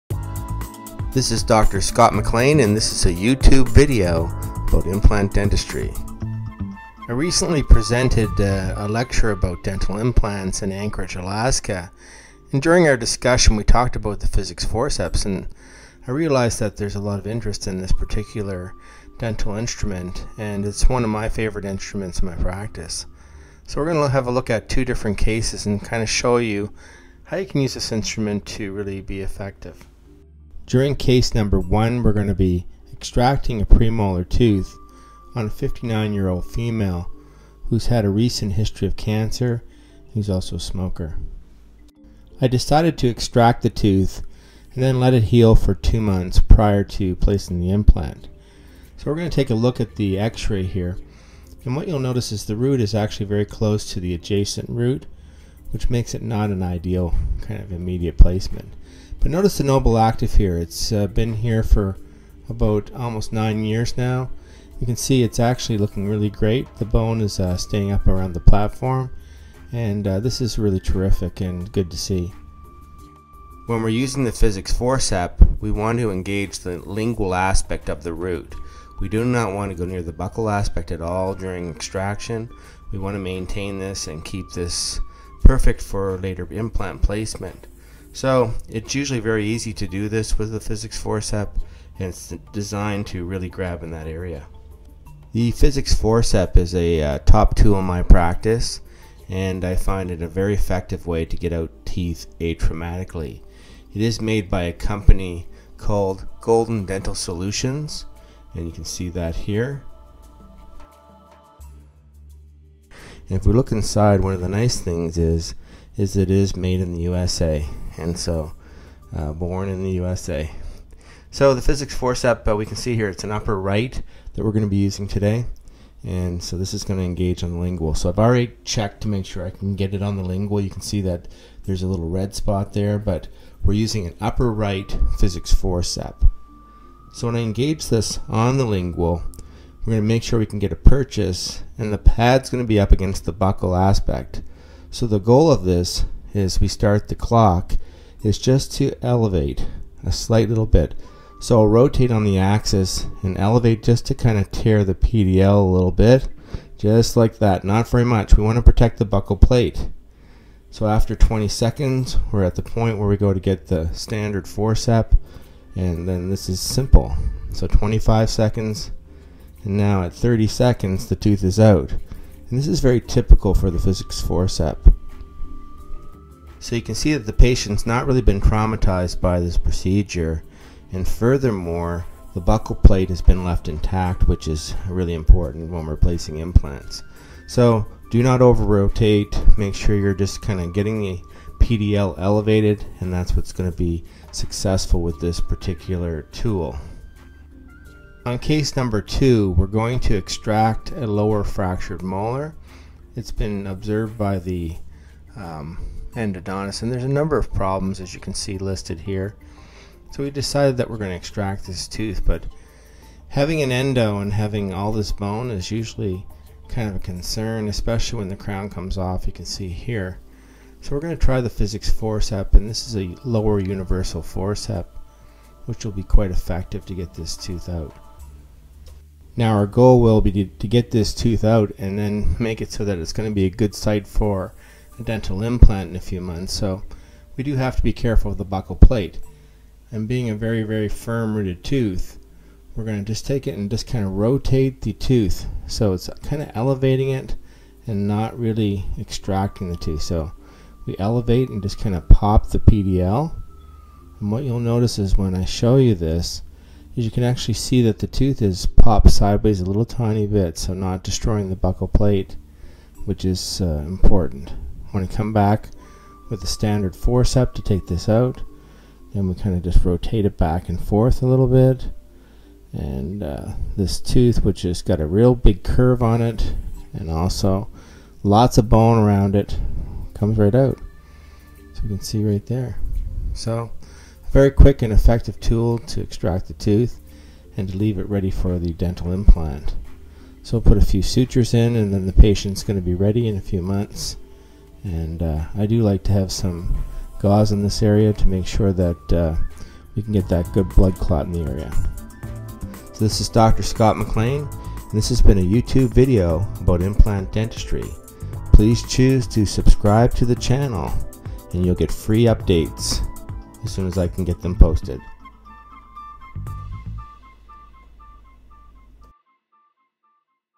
*Explicit Surgery - Atramatic Extraction - Preparation for Dental Implant
1,500,000 views on the youtube channel. The Physics Forcep is a very valuable tool for removing teeth prior to implant placement. This video is intended for dentists only. It is graphic in visual surgery.